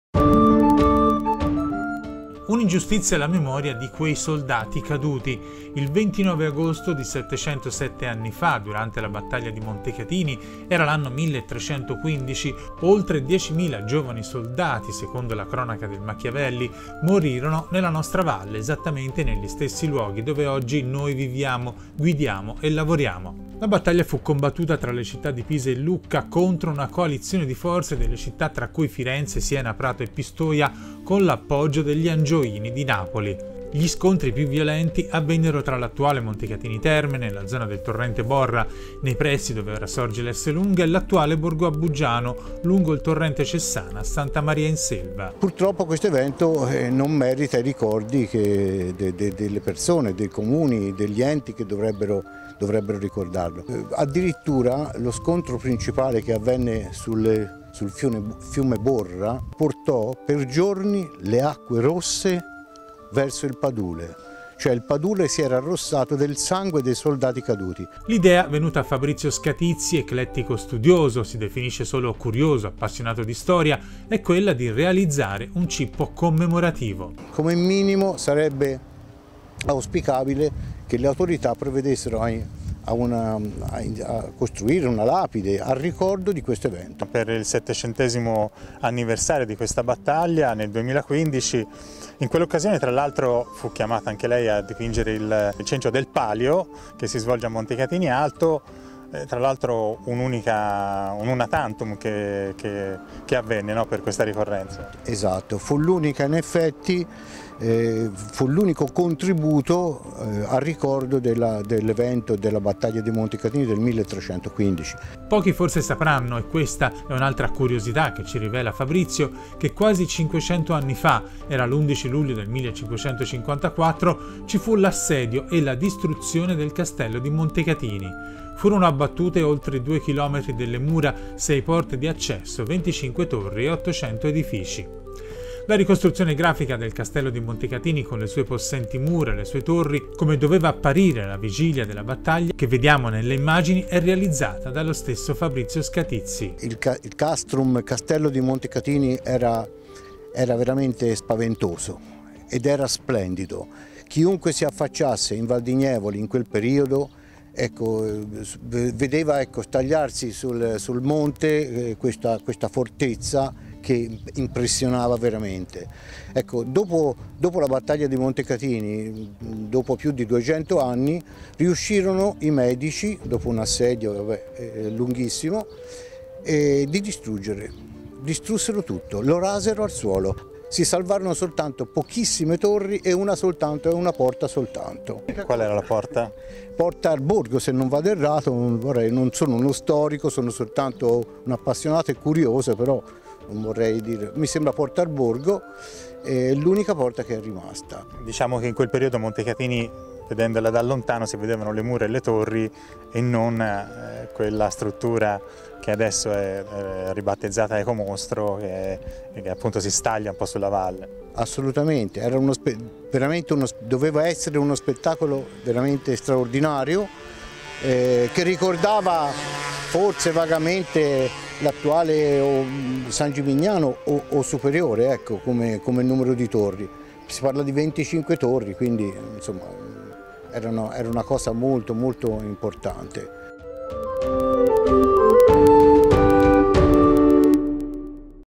Un cippo per i 10mila soldati morti
Un'ingiustizia alla memoria di quei soldati caduti. Il 29 agosto di 707 anni fa, durante la Battaglia di Montecatini, era l'anno 1315, oltre 10.000 giovani soldati (secondo la cronaca del Machiavelli) morirono nella nostra valle,  esattamente negli stessi luoghi dove oggi noi viviamo, guidiamo e lavoriamo. La battaglia fu combattuta tra le città di Pisa e Lucca contro una coalizione di forze delle città tra cui Firenze, Siena, Prato e Pistoia con l'appoggio degli Angioini di Napoli. La battaglia fu conseguente all'assedio del castello di Montecatini da parte dei pisani.Gli scontri più violenti avvennero tra l'attuale Montecatini Terme, nella zona del torrente Borra, nei pressi dove ora sorge l' Esselunga, e l'attuale Borgo a Buggianom lungo il torrente Cessana a Santa Maria in Selva.L'idea, venuta a Fabrizio Scatizzi, eclettico studioso, si definisce solo curioso appassionato di storia, è quella di realizzare un cippo commemorativo.Pochi forse sapranno, e questa è un'altra curiosità che ci rivela Fabrizio, che quasi 500 anni fa, era l'11 luglio 1554, ci fu l'assedio e la distruzione del Castello di Montecatini. Furono abbattute oltre due chilometri delle mura, 6 porte di accesso, 25 torri e 800 edifici …La ricostruzione grafica del castello di Montecatini con le sue possenti mura e le sue torri, come doveva apparire alla vigilia della battaglia è sempre di Fabrizio Scatizzi